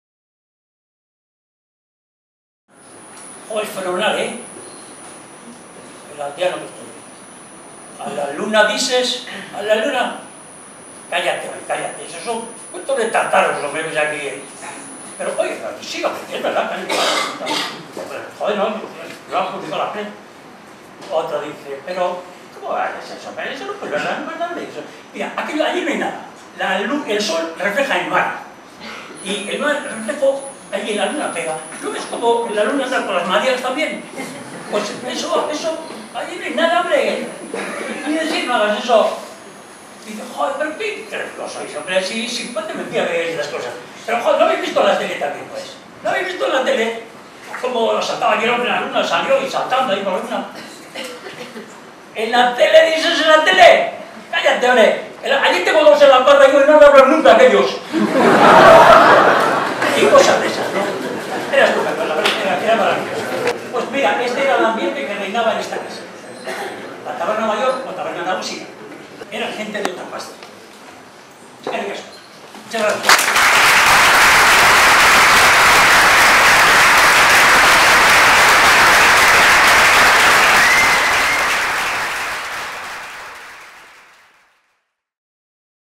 Basauri // Pleno San Miguel. Chiste por Patxi Ibarra
Basauri // Chiste contado por el vecino de San Miguel, Patxi Ibarra, en el pleno histórico celebrado en la Taberna Mayor. Más info